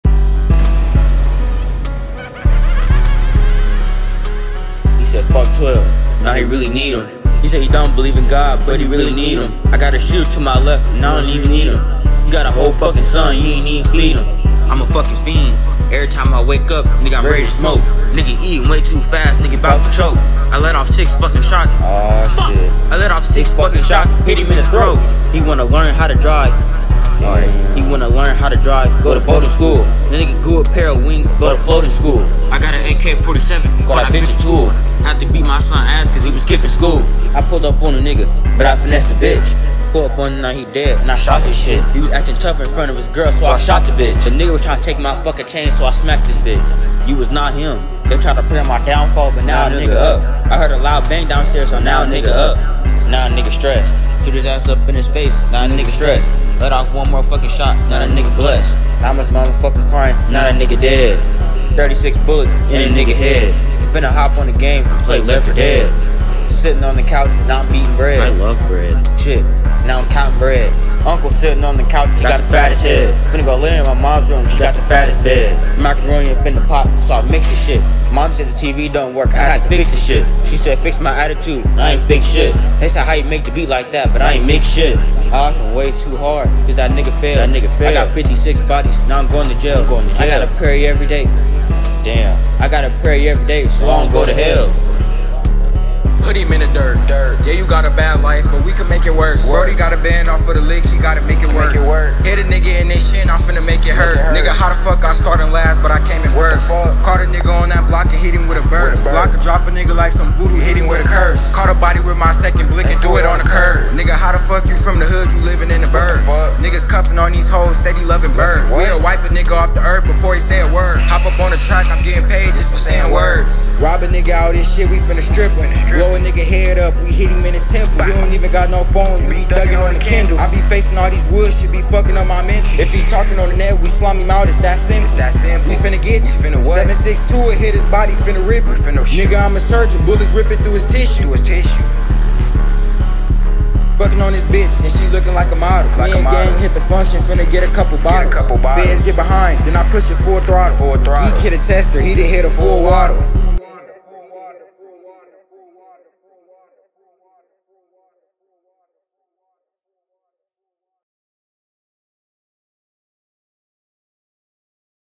THANK YALL FOR 800 SUBS
firs verse is me second one is my bruda nell

i dont rap chill its only cause a nigga hit 800 u wont hear me on another track😂

happy halloween 🎃